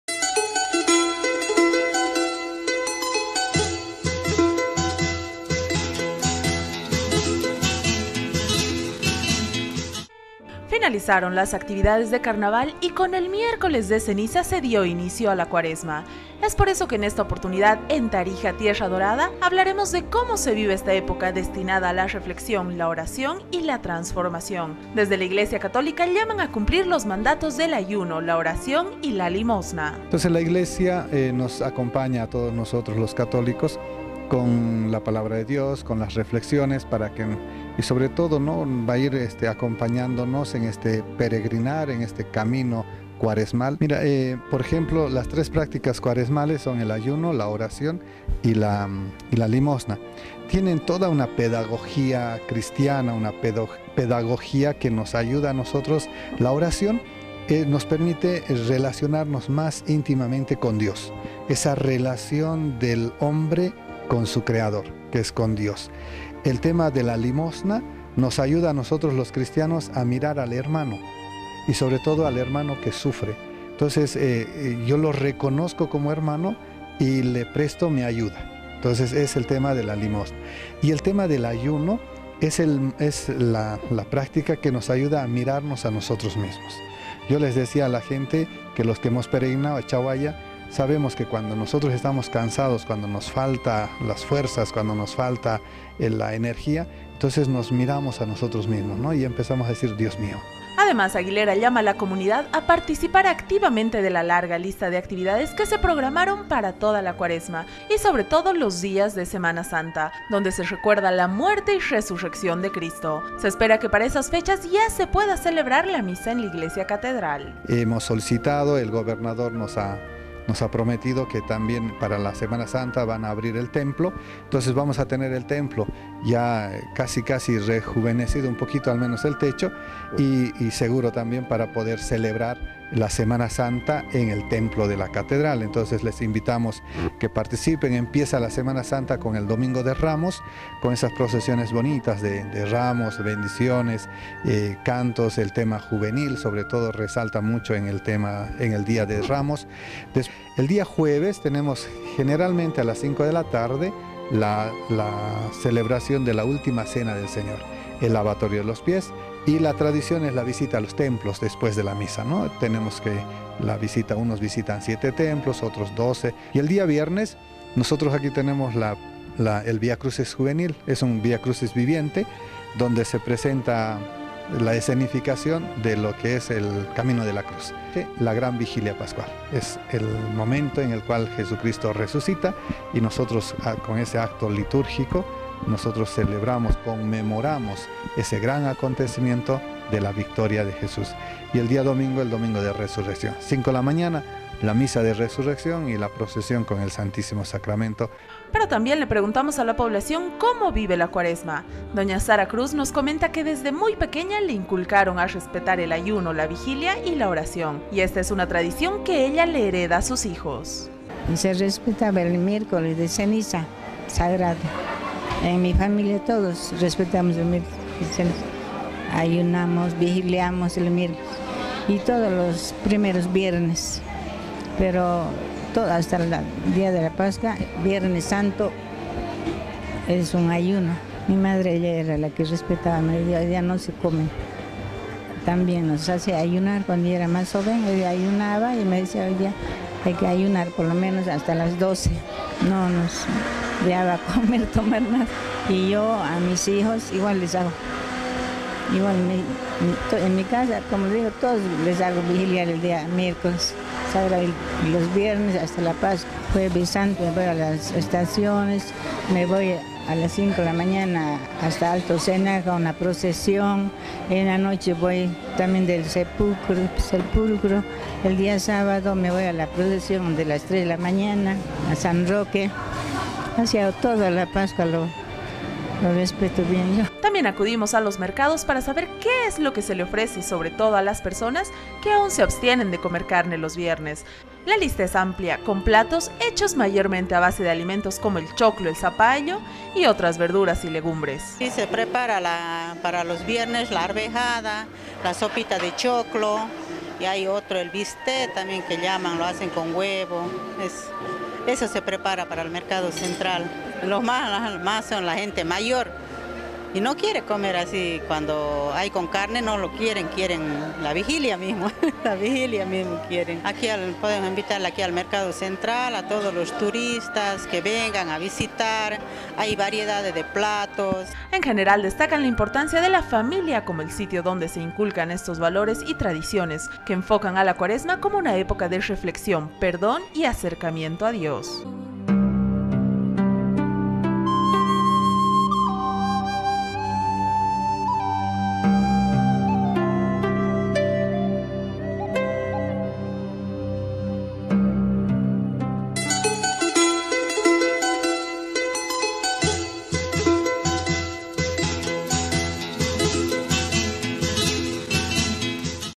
LA CUARESMA, UNA ÉPOCA DE ACERCAMIENTO A DIOS
El Miércoles de Ceniza da inicio a la Cuaresma, en esta oportunidad hablaremos de cómo se vive esta época destinada a la reflexión, la oración y la transformación.